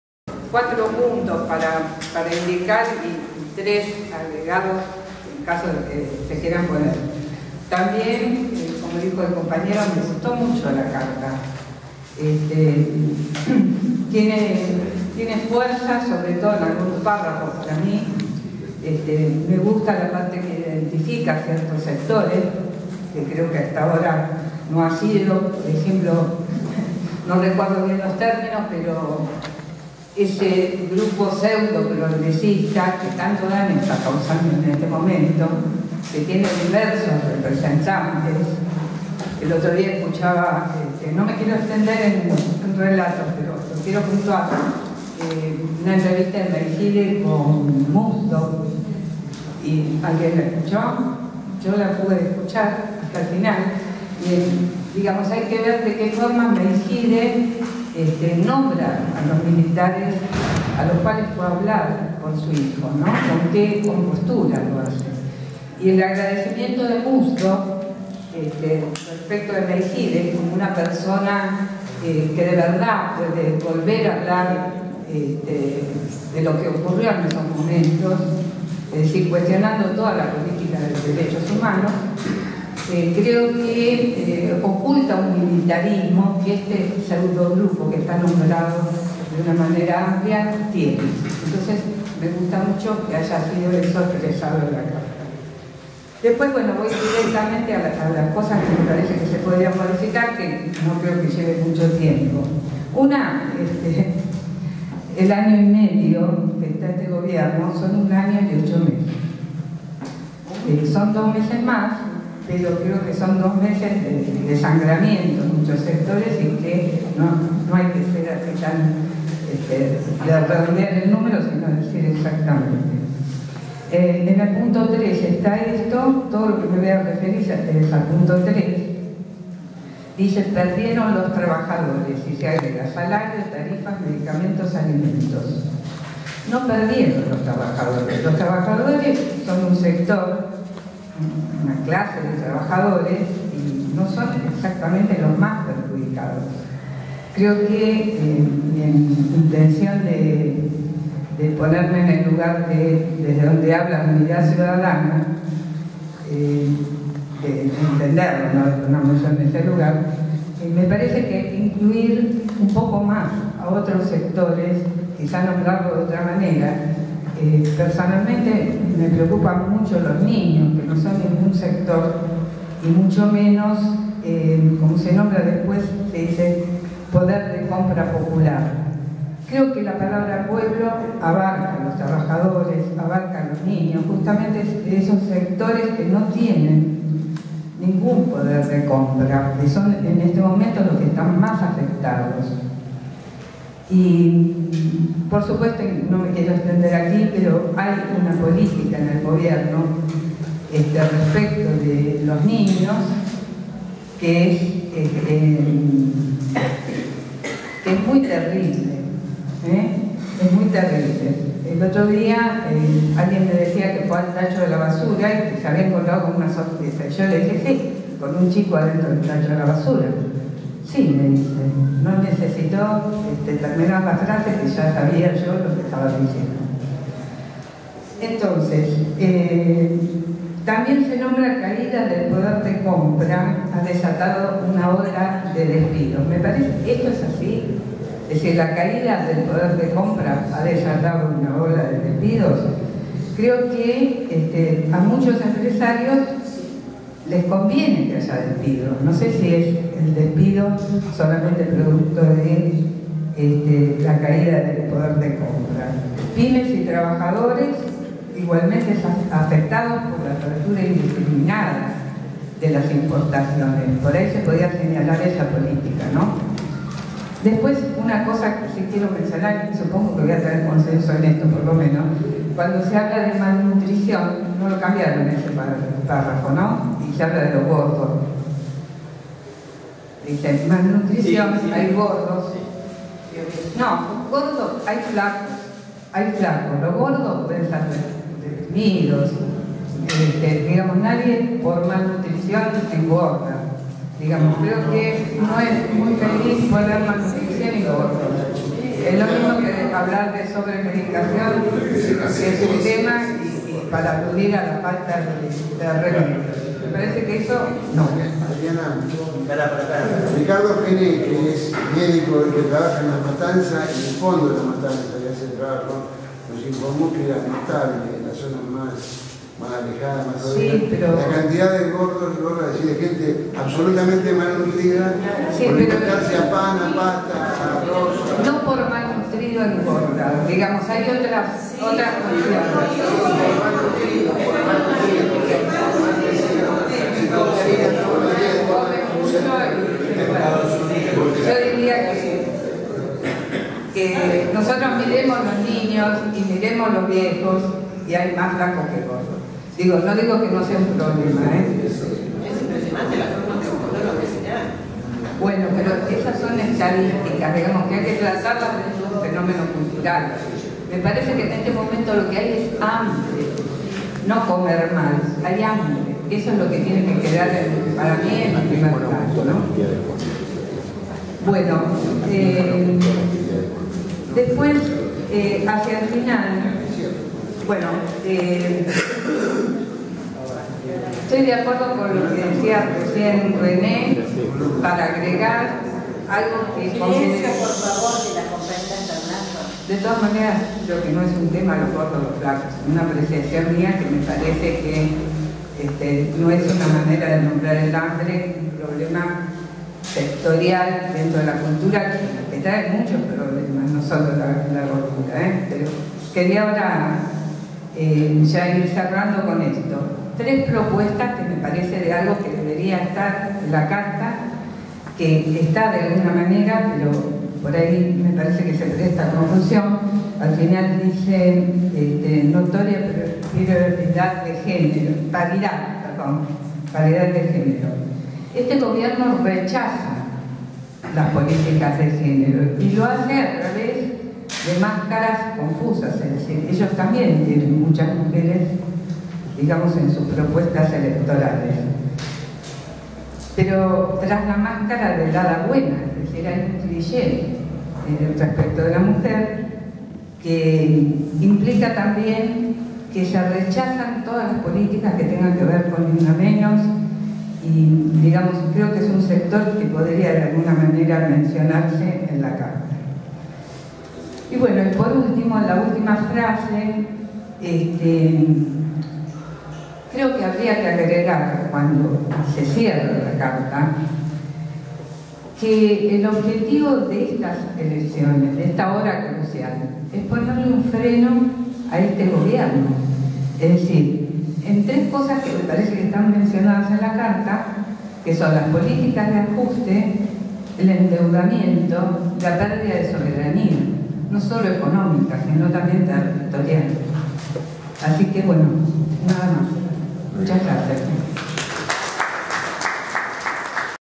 02CA Adriana Fernández 050817
Asamblea del sábado 5 de agosto en la Facultad de Ciencias Sociales.(UBA). Aportes y discución a la Carta Abierta/ 24 "ANTE UNA HORA CRUCIAL"